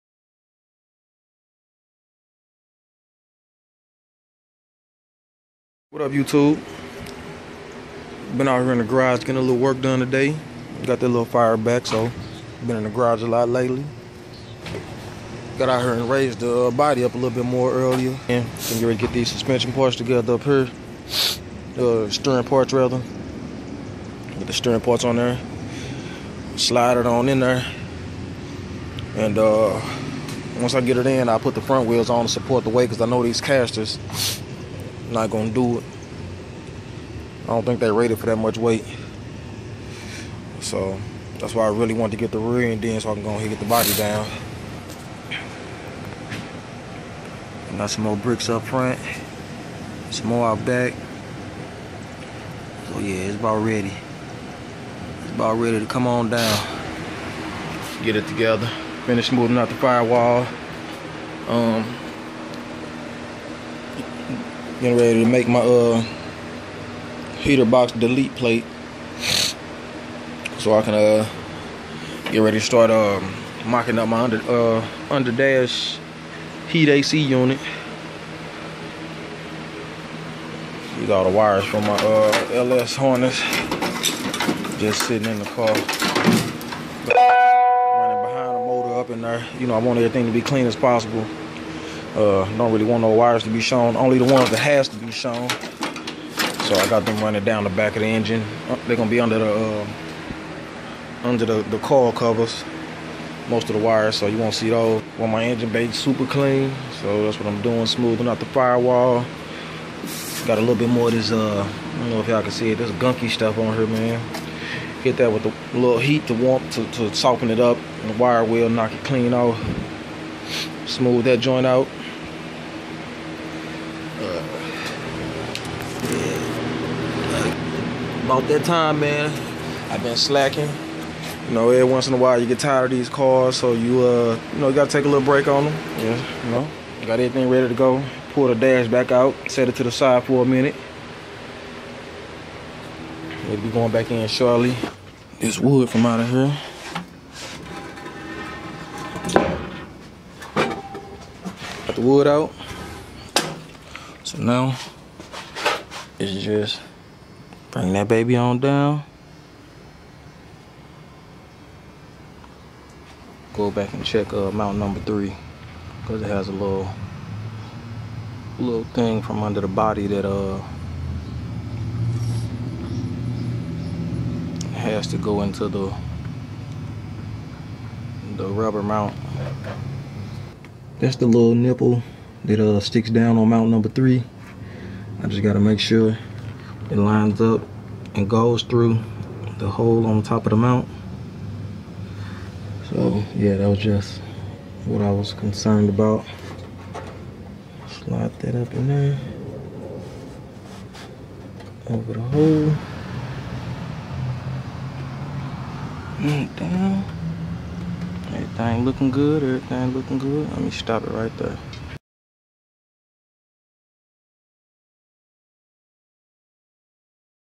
Cutlass Update 3-28-16
Here's an old vid I recorded a while back & never uploaded. I'm posting these to help bring you to my current stare with the build without skipping anything.